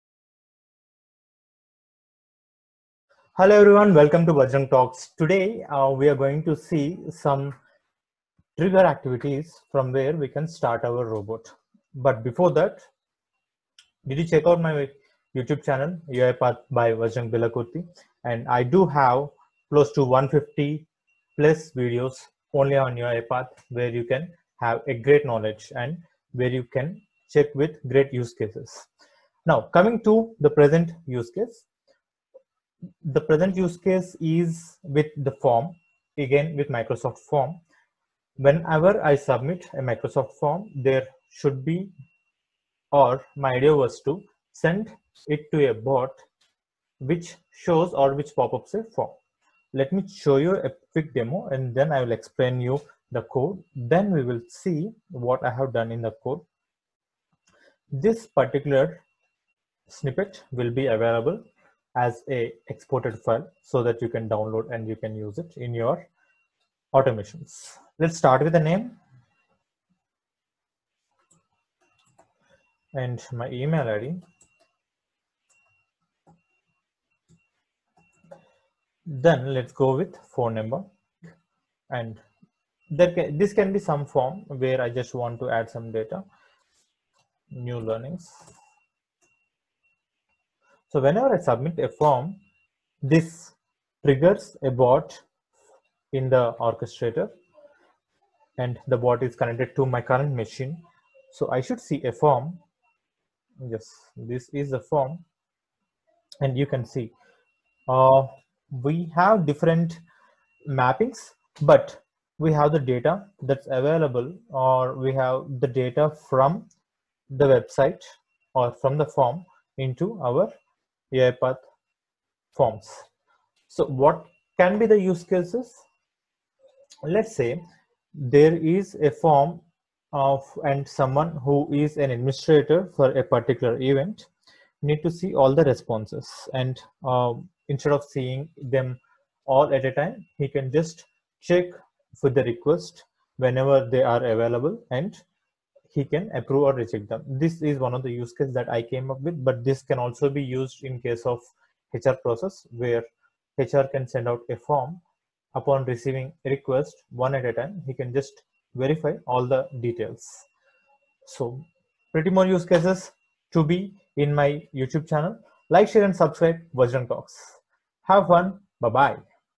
UiPath Demo Trigger UiPath Bot on Microsoft Form Submission | View details In UiPath Dynamic Forms
Check demo On how to trigger UiPath robot on Microsoft from submissions